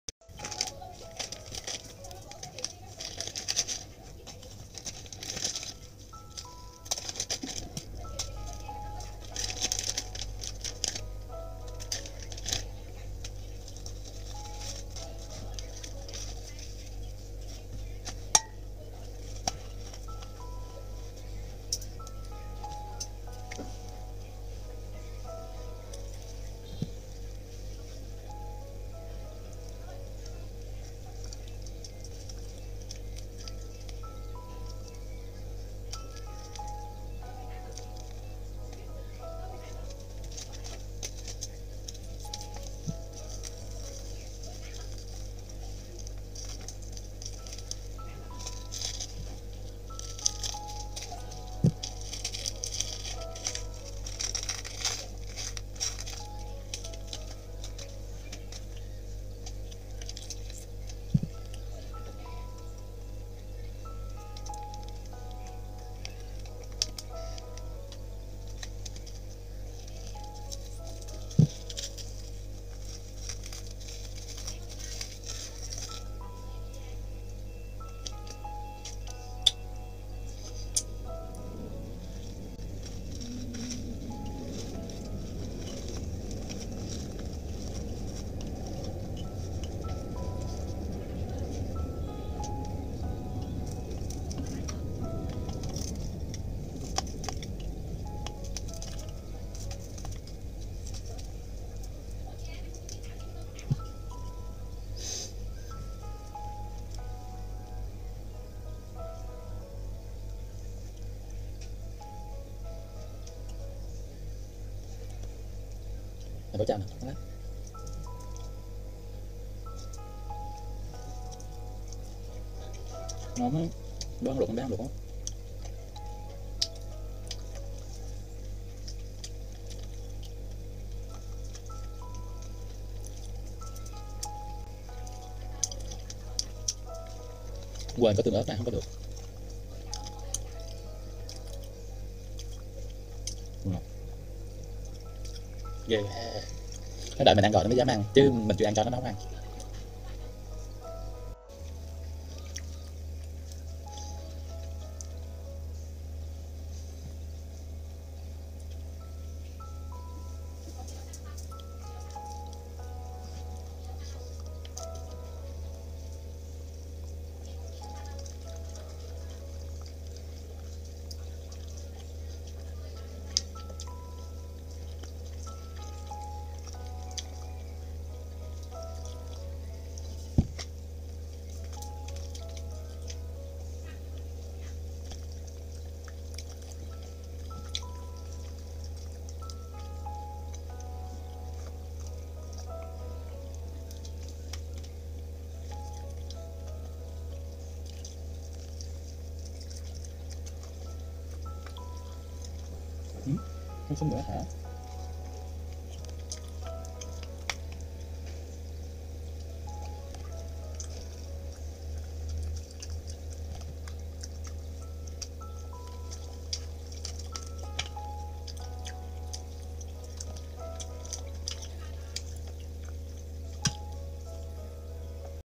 Cách ăn món bánh tráng bơ tây ninh
Cách làm món bánh tráng bơ nhanh gọn mà ăn ngon nhất. Mới mua online từ fb Út Nguyễn thạc sĩ.